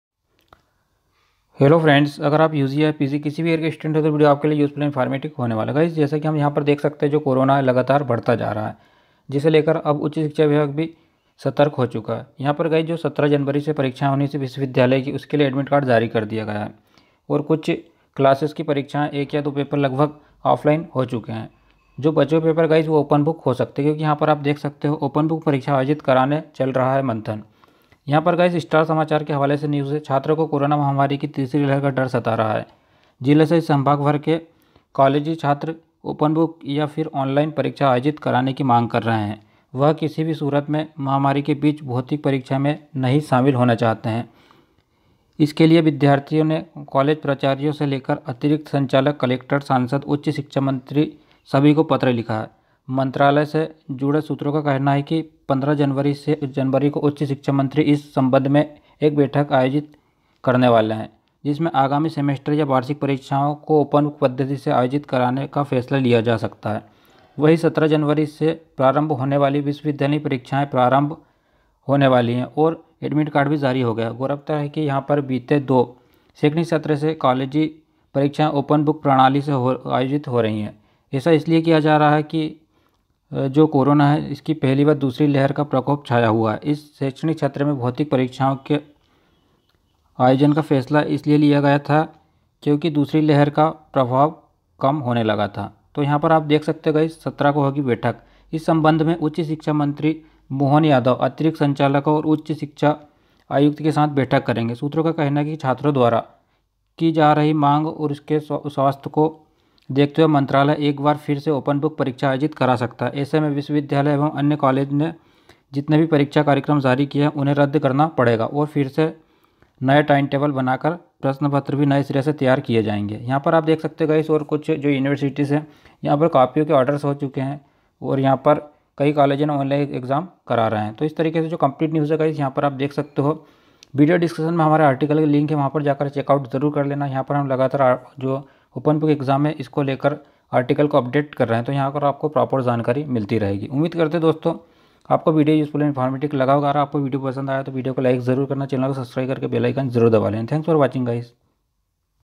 Latest News on open book exams : ओपेन बुक परीक्षा आयोजित कराने चल रहा मंथन
नमस्कार दोस्तों 17 जनवरी  से होनी है विश्वविद्यालयीन परीक्षाएं जिसको लेकर जारी किया एडमिट कार्ड जारी कर दिया गया है लेकिन कोरोना का संक्रमण कहा थमा है लगातार बढ़ रहा है यदि सरकार अभी भी अपनी मनमानी करती है तो नतीजा ख़राब हो सकता है क्योकि जिस रफ़्तार से कोरोना बढ़ रहा है अकड़े चौकाने वाले है यदि उच्च शिक्षा विभाग अभी भी अपनी जिद्द पर रहा की ऑफलाइन एग्जाम करायेगे तो आप समझ सकते हो परिणाम क्या हो सकते है मध्य प्रदेश में लगभग 32 यूनिवर्सिटी है जिसमे लाखों छात्र पढ़ते है यदि ऑफलाइन एग्जाम होते है तो आप समझ सकते है की कितना बढ़ा खतरा होगा सरकार छात्रों के साथ कोई भी रिस्क नहीं ले सकती जैसा हम पिछले वर्षो में भी देख चुके है साल भर स्कूल बंद रहे और ऑनलाइन एग्जाम हुए तो दोस्तों आप अंदाजा लगा सकते है की यदि Covid के केस इसी रफ़्तार में बड़े जैसे अभी बढ़ रहे है तो एग्जाम होना संभव नहीं सरकार के पास ओपन बुक के अलावा कोई दूसरा विकल्प नहीं होगा।